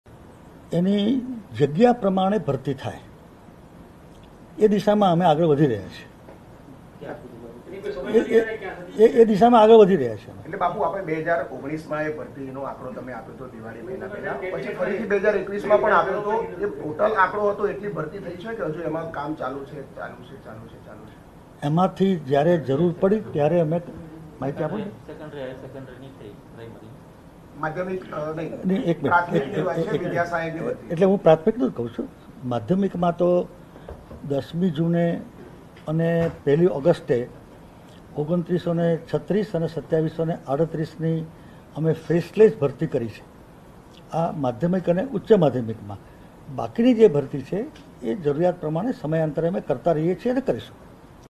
ટેટ 2 વિધ્યાસહાયક ભરતી બાબતે ચોથીજાગીરે જોરદારસવાલ અને સ્પષ્ટ જવાબ માગતા બાપુ સલવાનાં.આભાર પત્રકાર
ટેટ 2 વિધ્યાસહાયક ભરતી બાબતે ચોથી જાગીરે જોરદાર સવાલ અને સ્પષ્ટ જવાબ માગતા બાપુ સલવાનાં..